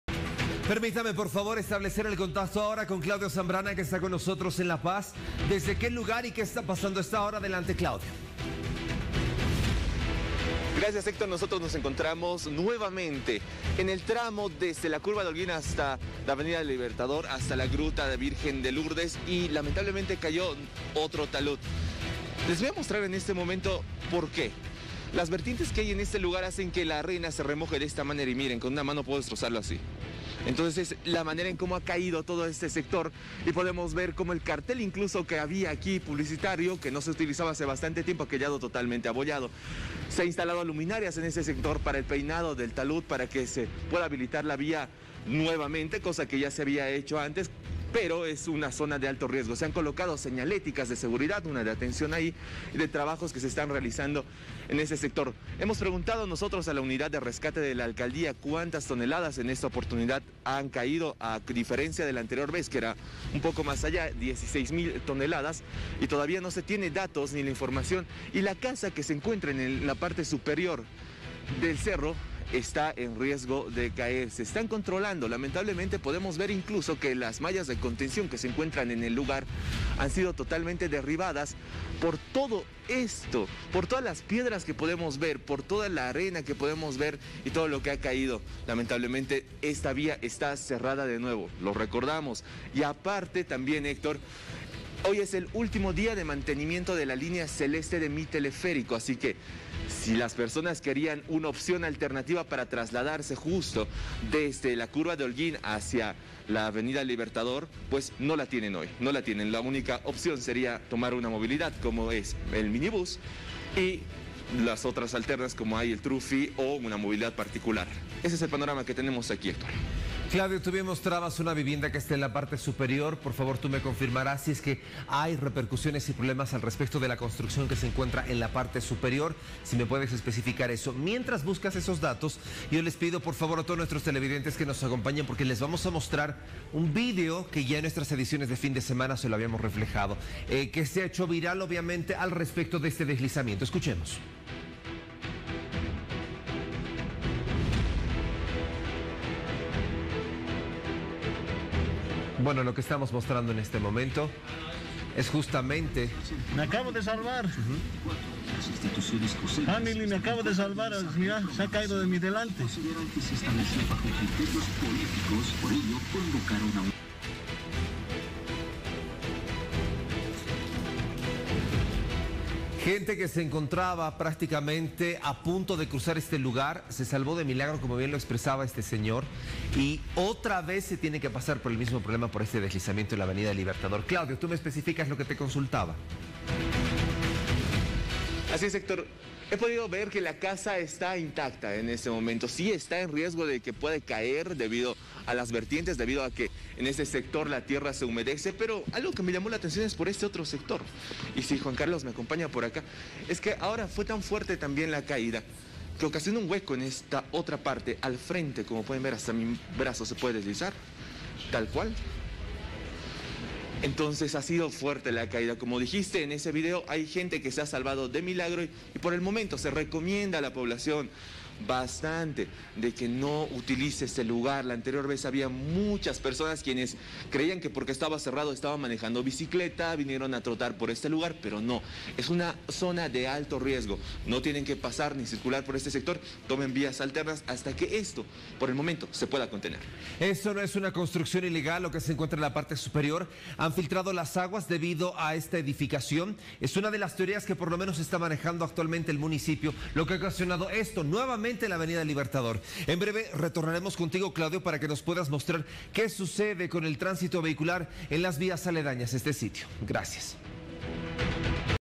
(U. Móvil) Nuevo deslizamiento en la avenida del Libertador
Nuevamente se reportó un deslizamiento en la avenida del Libertador debido que el terreno se encuentra con filtraciones de agua. Malla de contención que colocaron tras el deslizamiento anterior, terminó destruida.